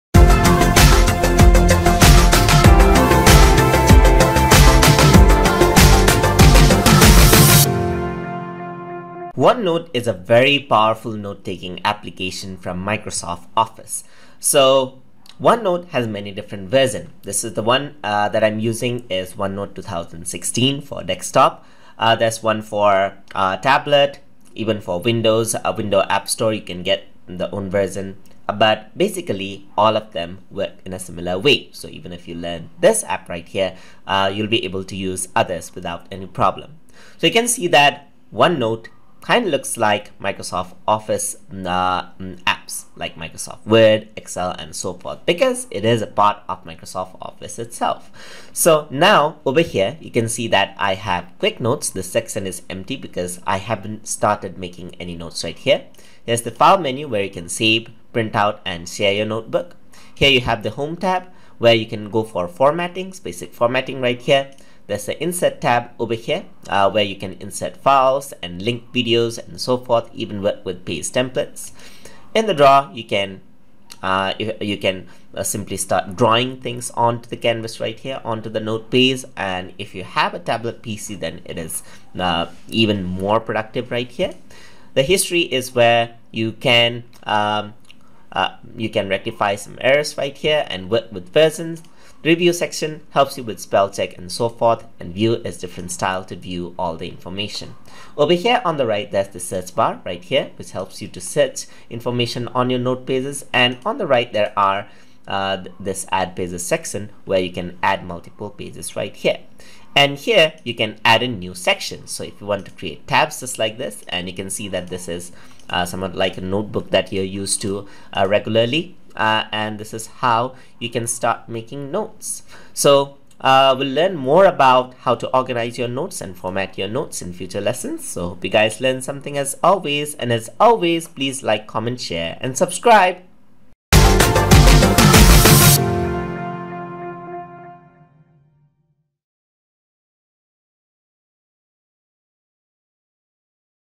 MS OneNote Tutorial - Lesson 1 - OneNote Interface
In this tutorial, we will be discussing about the Interface of MS OneNote.

Learn the basics of using MS OneNote, including how to work with pages, work with templates, work with different file formats, design using good layout and colours and get everything ready for proper note taking.

This lesson has been made using MS OneNote 2016 but is applicable to older versions and newer versions of OneNote as well.

If you like this video, here's our entire playlist of MS OneNote tutorials: